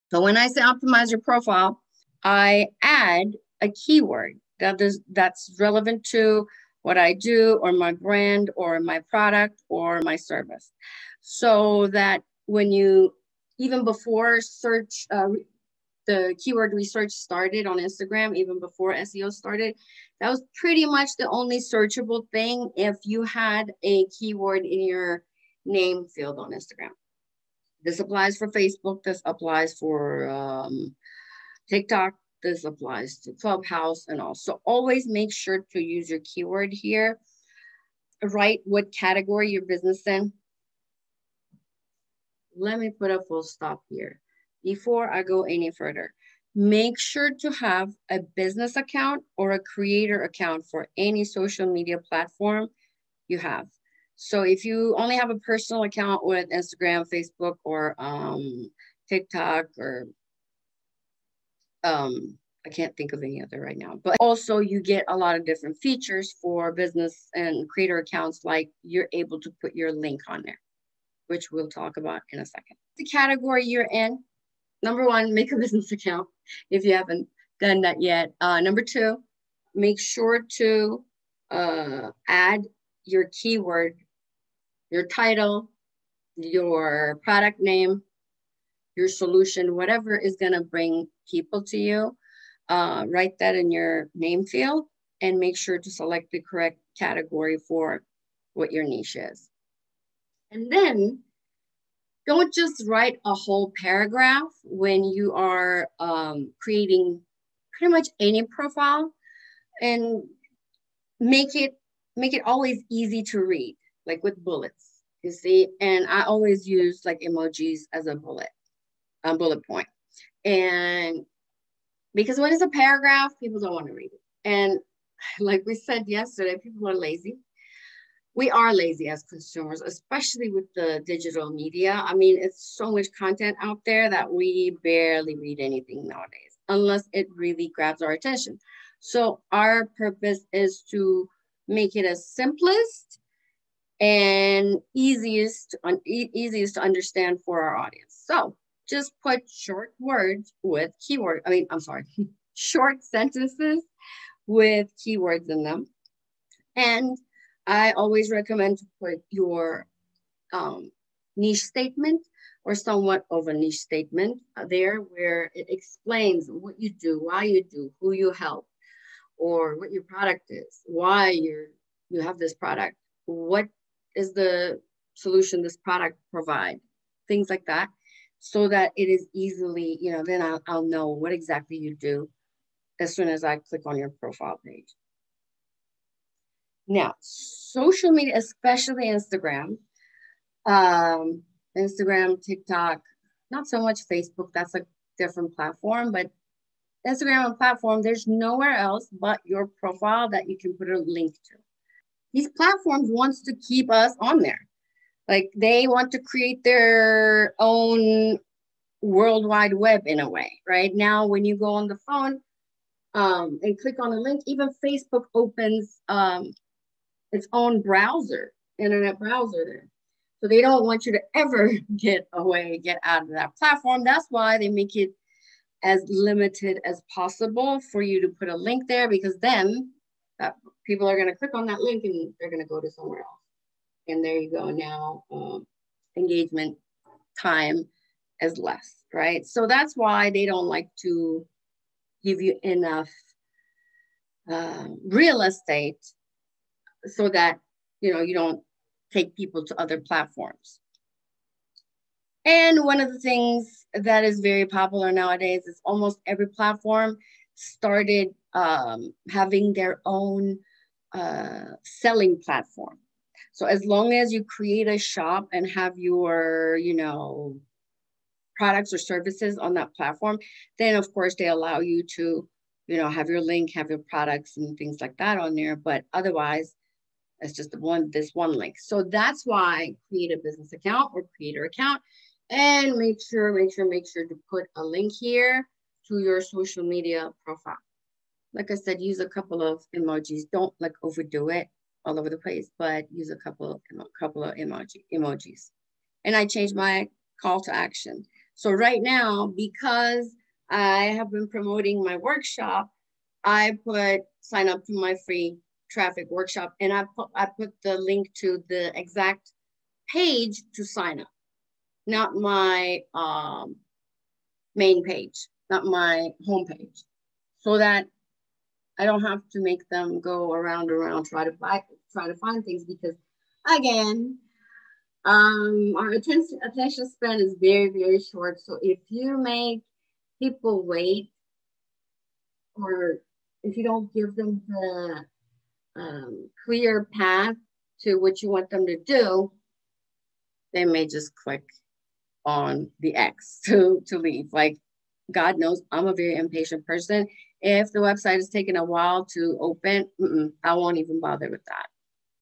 How to Optimize Social Media Profiles 2021
In my video How to Optimize Social Media Profiles 2021, I am showing the best practices for creating the perfect social media profile to bring you more clients.
If you want to learn How to Optimize Social Media Profiles 2021 watch this video and let me know what was the biggest takeaway in the comments.